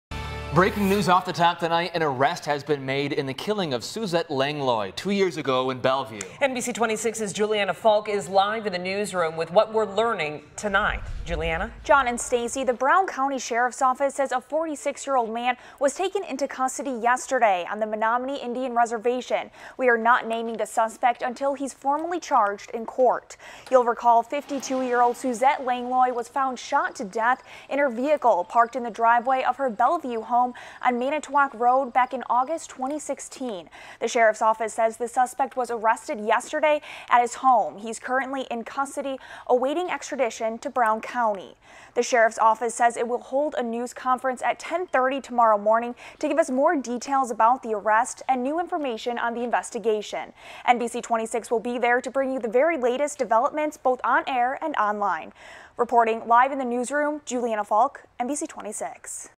Arrest made in the Suzette Langlois homicide investigation
An arrest warrant for 1st Degree Intentional Homicide was issued for a 46-year-old suspect.